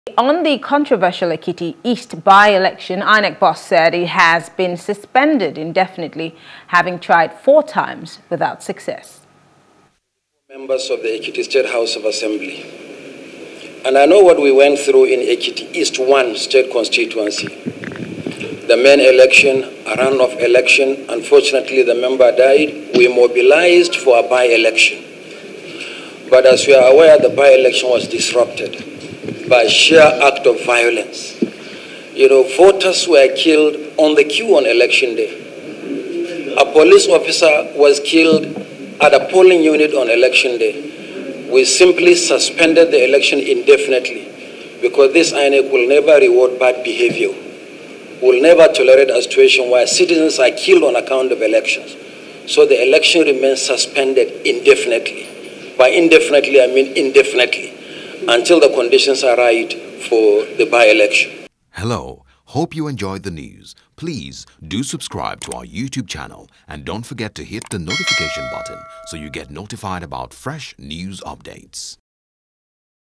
Ekiti East Bye-Election: INEC Suspend Poll Indefinitely | NEWS
On the controversial Ekiti East bye-election, INEC boss said it has been suspended indefinitely having tried four times without success.